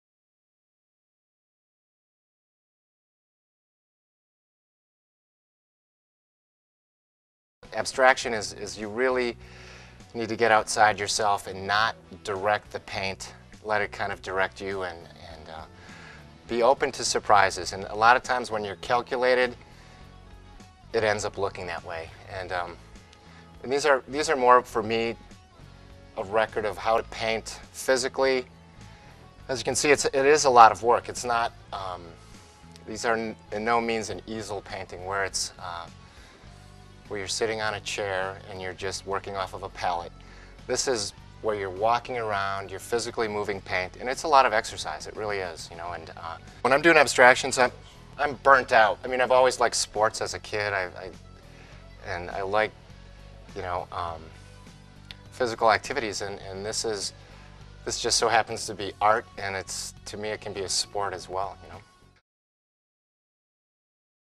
Charles Dwyer Abstraction Short.mov
Charles Dwyer discusses his approach to abstraction.  Filmed and Edited by Jimmy Sammarco.